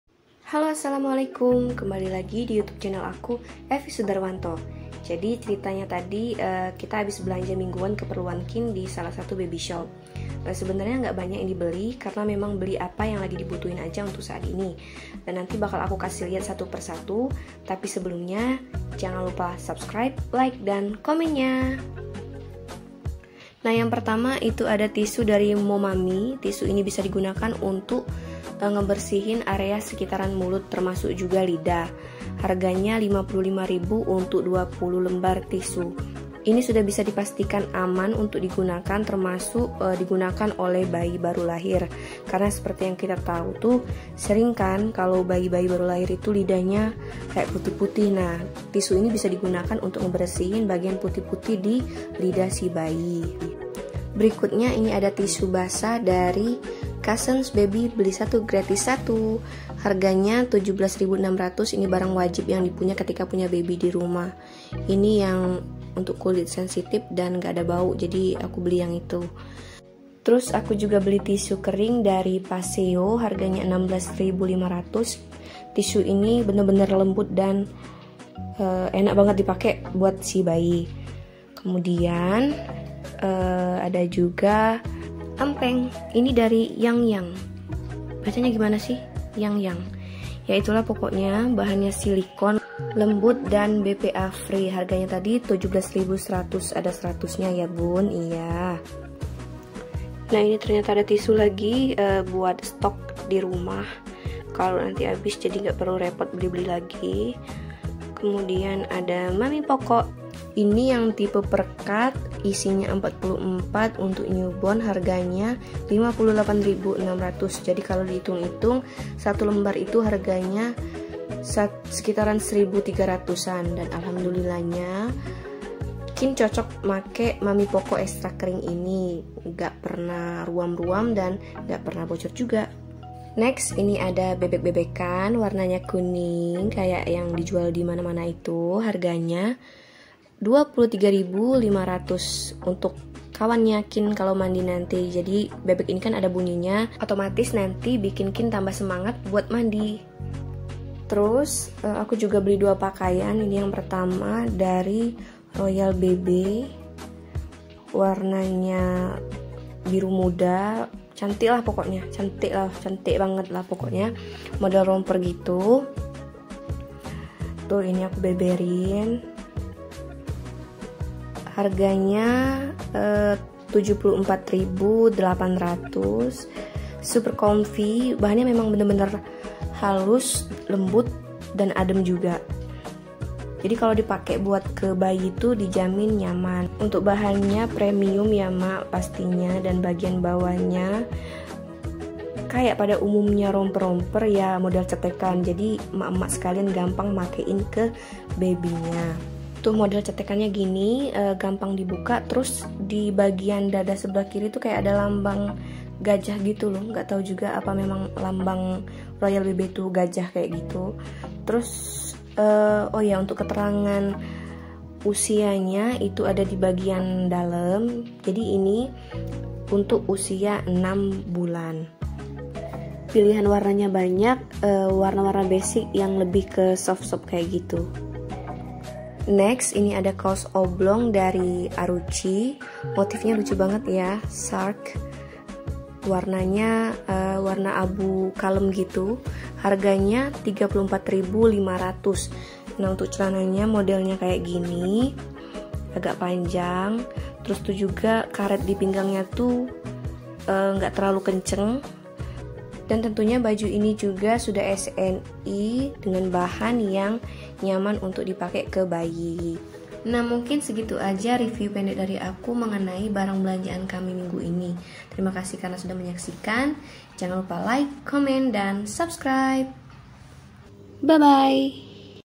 BELANJA PERLENGKAPAN BAYI BARU LAHIR // NO KALAP!
Beberapa saran dari aku sebelum belanja.

1. Belanja karena butuh bukan karena lucu, tahan mata, list belanjaan sebelum belanja.

2. Usia dua bulan, anak lebih fokus ke bunyi dan warna, jadi cari mainan yang bisa mengeluarkan bunyi dengan warna yang mencolok agar menarik perhatian si kecil. Mainan ini juga bisa menstimulasi perkembangan motorik halusnya.

3. Jangan lupa belanja kasa atau tisu khusus  untuk membersihkan bagian sekitaran mulut. Perawatan seperti gusi dan lidah bayi baru lahir penting dilakukan sehari dua kali agar tidak mengganggu pertumbuhan gigi dan tidak menyebabkan munculnya jamur. Aktivitas ini juga diharapkan bisa melatih si kecil agar terbiasa membersihkan gigi sedari kecil.

4. Untuk baju, pilihlah baju dengan bagian leher yang gampang dipakaikan ke bayi, apalagi untuk bayi baru lahir, lebih gampang dengan baju berkancing depan.

5. Untuk empeng sebenernya gak terlalu dianjurin, mengingat efek dari pemakaiannya. Tapi kalau terpaksa harus beli kayak aku karena beberapa alasan sebaiknya cari empeng yang murah aja dulu, karena cocok-cocokan juga sama kayak dot. Takutnya nanti udah beli yang mahal taunya dedek bayi gak mau make, jadinya mubadzir. kan sayang mak.

Terimakasih :)